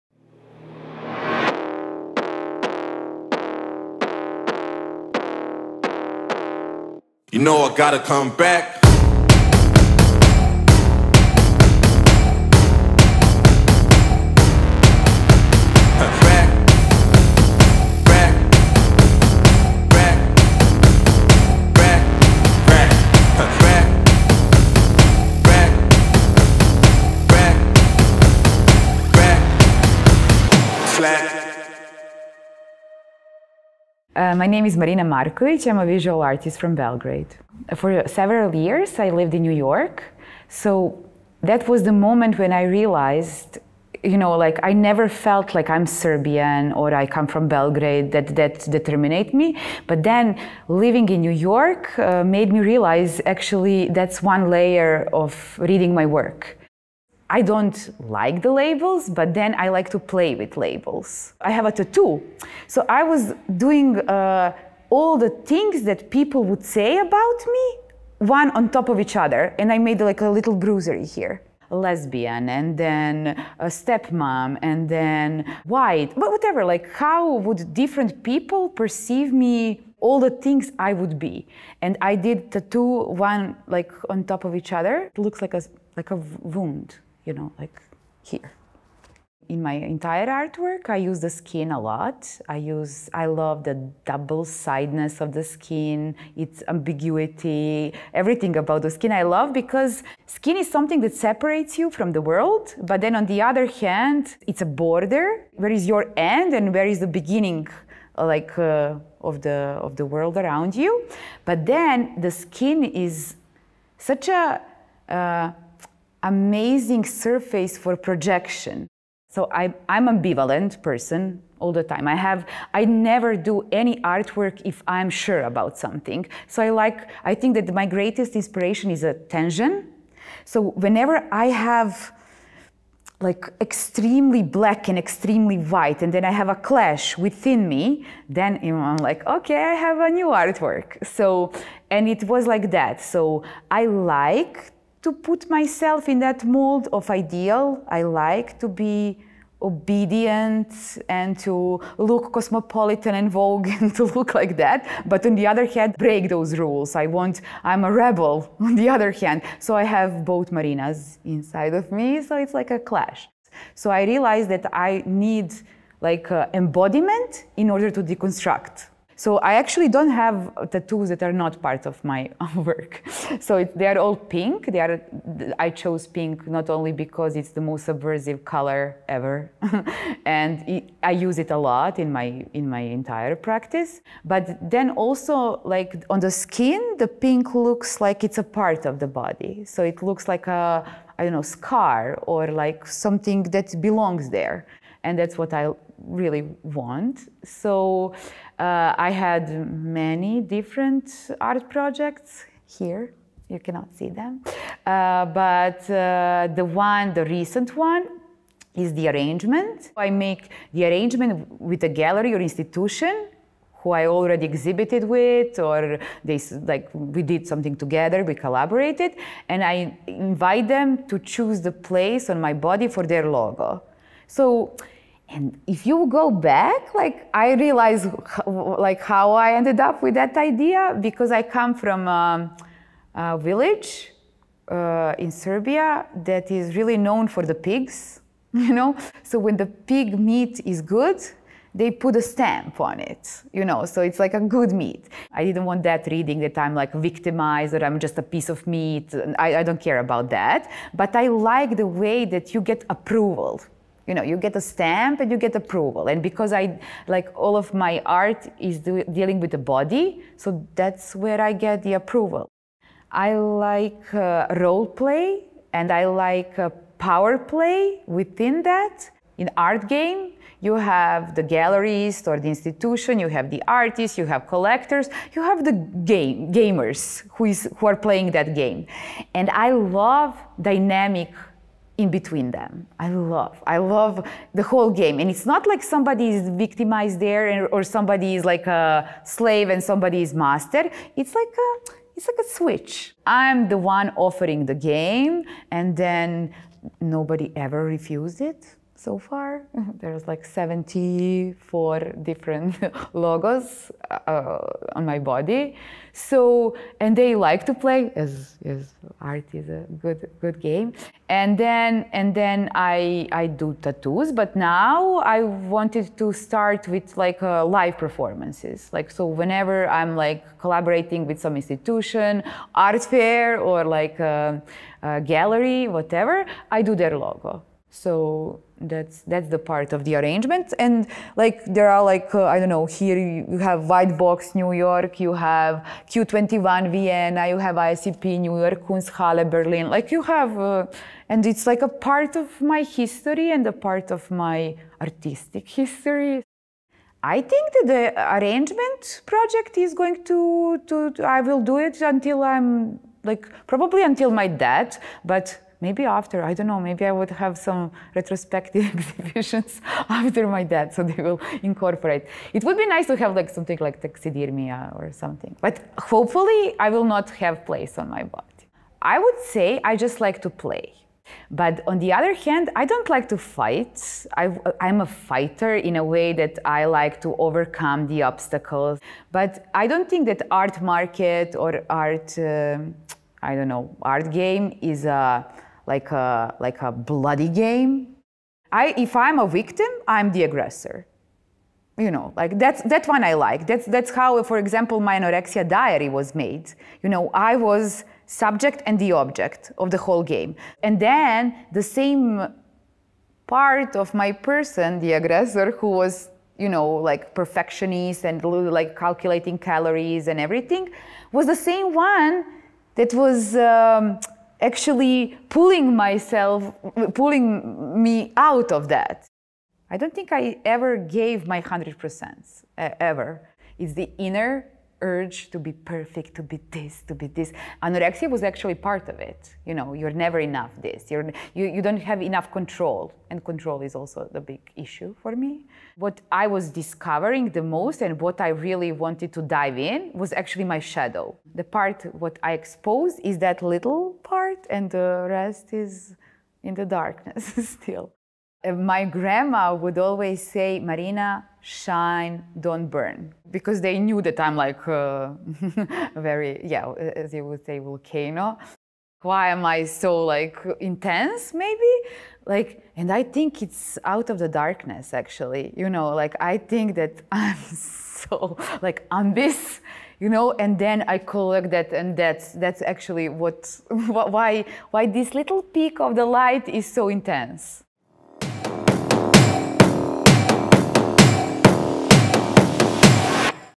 MARINA MARKOVIC in conversation with Denis Leo Hegic & Jan Gustav Fiedler I UNBLOCK FAIRARTFAIR
Wie ein Wirbelsturm zieht sie gerade durch Berlin: die Künstlerin Marina Markovic spielt gerne mit dem "art game" und verbiegt die Spielregeln. Mit ihrer Arbeit "The Arrangement" geht Markovic Verträge mit Institutionen, Galerien und Kunsträumen ein, und bietet eine Kollektion eigener Körperstellen für die Positionierung von Logos, die auf die Haut tätowiert werden. Die Arbeit ist ein fortlaufendes Projekt, das morgen Abend bei unserer Eröffnung in einer weiteren Performance gezeigt wird.

"Das Arrangement werde ich wahrscheinlich bis zu meinem Tod machen. Hoffentlich bleiben dann keine Stellen mehr übrig." sagt Markovic im Gespräch mit unseren Kuratoren Denis Leo Hegic und Jan Gustav Fiedler.

UNBLOCK FAIRARTFAIR Berlin Lichtenberg
Save the date:
10-11-12 & 17-18-19 September 2021
Unblock Art Fair Berlin

Production: Supermarket Lab 2021
Camera, Sound: Jan-Holger Hennies
Editing: Elizaveta Kapustina
Berlin, September 2021